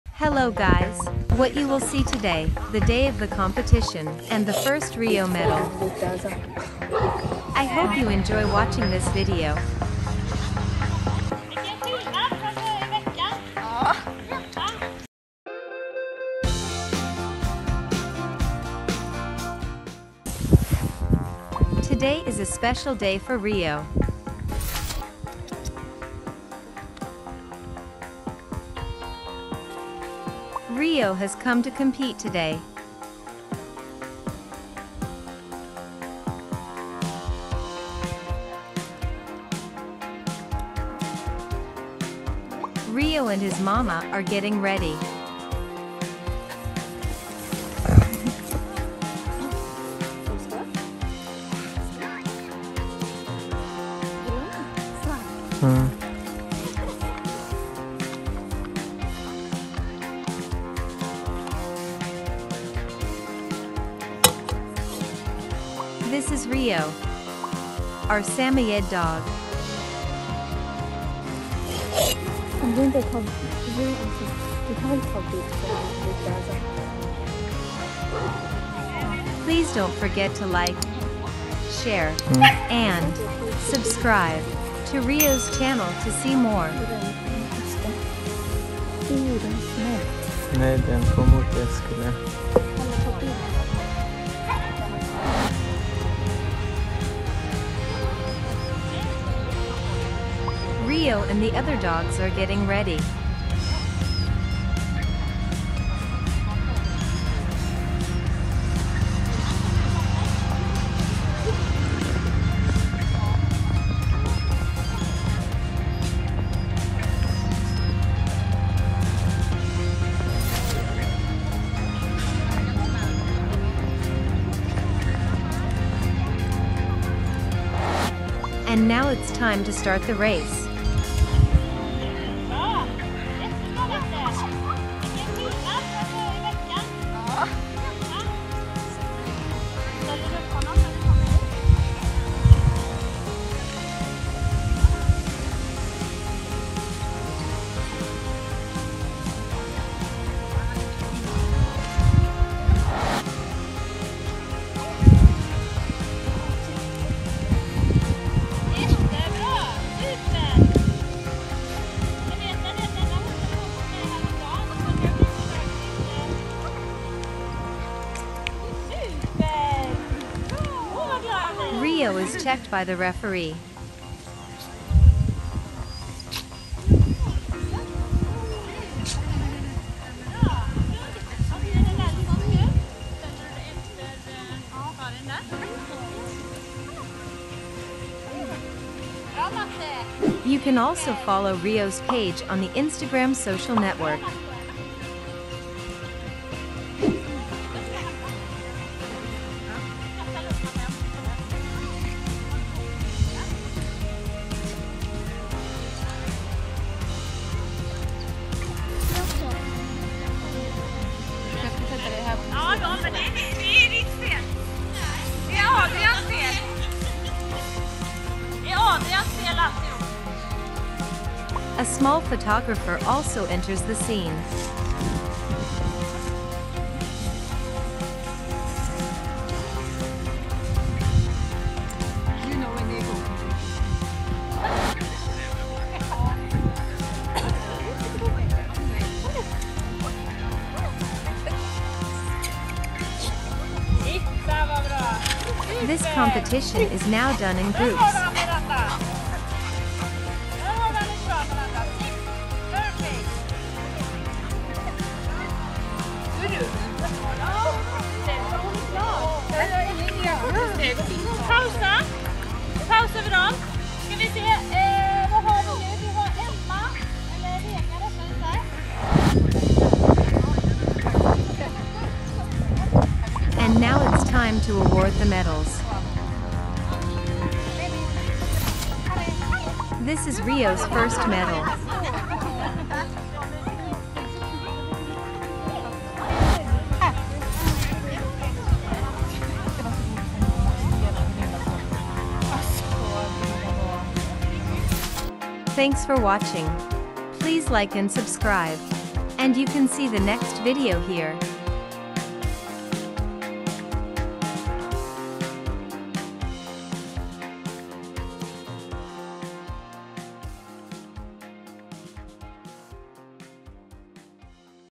The first samoyed dog race and medal 🎖️🐶
In this section, you can see that we participated in the dog competition. 😍
In the beginning Rio the Samoyed dog and his mother are preparing before the race. 😌🐶

Then Rio the Samoyed puppy and his mother start the race. ☺️
And finally Rio the Samoyed dog wins his first medal. 😍🏅🎖️🐶

00:00 Intro
00:20 Preparing for the dog race
02:07 The start of the dog race
04:29 Giving medals to dogs
04:54 Next Video